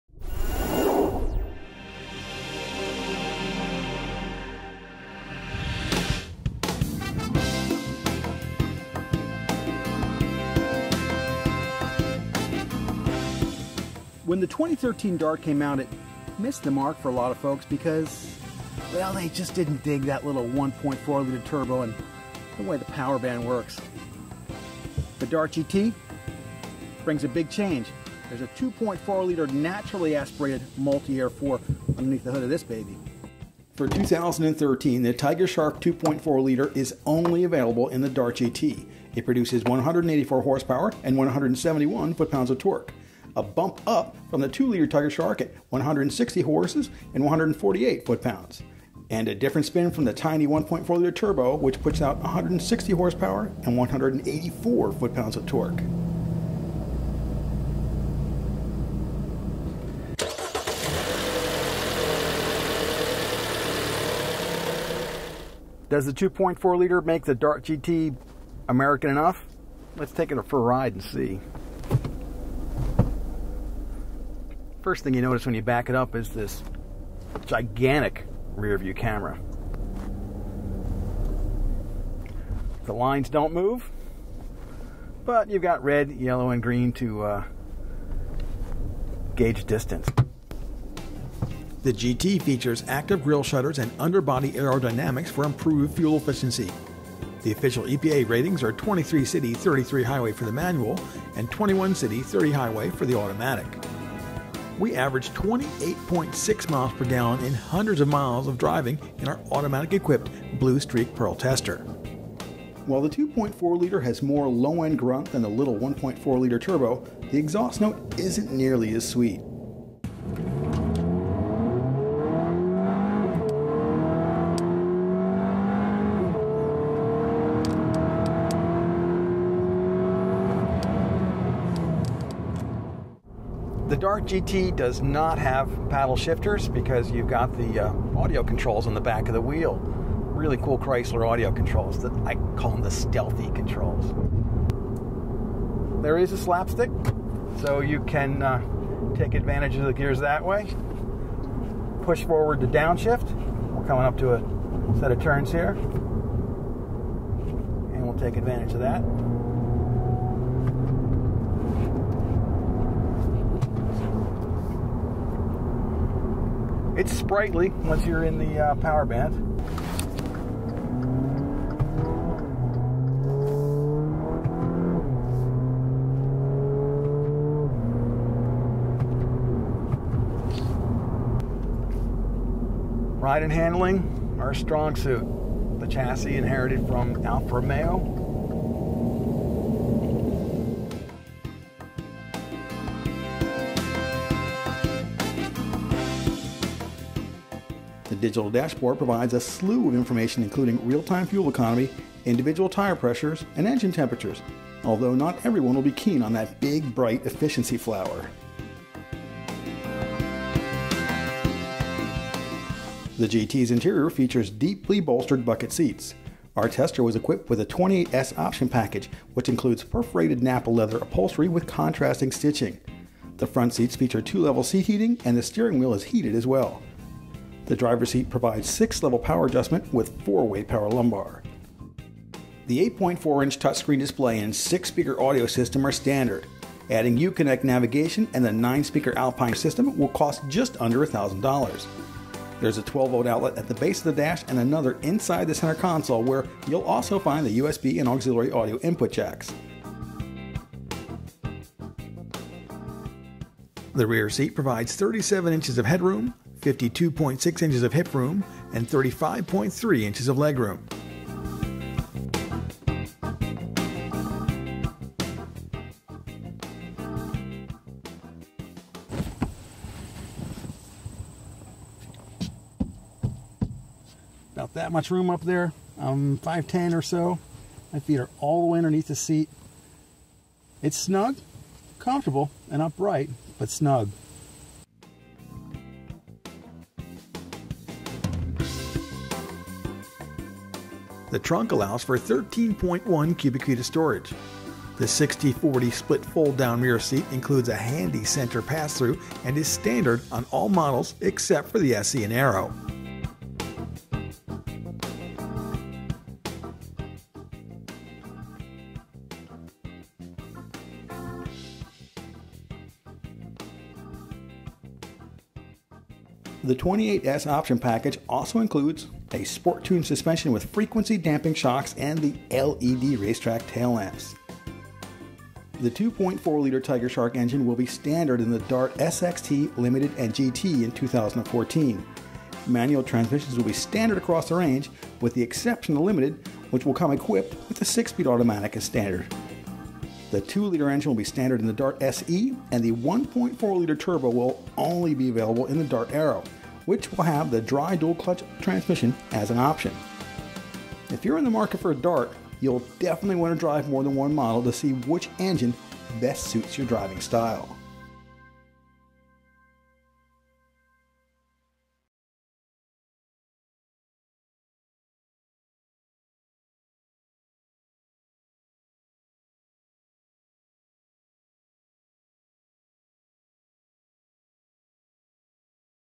2013 Dodge Dart GT Test Drive & Compact Car Video Review
When the 2013 Dodge Dart debuted, it missed the mark for many consumers due to the peaky power band that stems from the 1.4 liter turbocharged engine. The Dodge Dart remedies that with a 2.4 liter naturally aspirated 4-cylinder engine under the hood. The Tiger Shark engine is only available in the GT model and produces 184 horsepower and 171 lb-ft. of torque.

The 2014 Dodge Dart GT features active air shutters and underbody aerodynamics to improve fuel efficiency. Equipped with a manual transmission you can expect 23 MPG in the city and 33 MPG on the highway. Fuel economy slumps a bit with the automatic transmission at 21 MPG in the city and 30 MPG on the highway.

The chassis was inherited from Alfa Romeo and the result is a nice ride. The interior features deeply bolstered bucket seats. This tester was equipped with the 28S Order Packing which includes: larger 18-inch wheels, LED Taillights, Sport Suspension, and a host of color matched body parts. The 8.4-inch touch screen display is standard but adding the Uconnect system and upgraded audio will cost just under $1000.

If you are in the market for a compact sedan then you will definitely want to drive the 2014 Dodge Dart GT.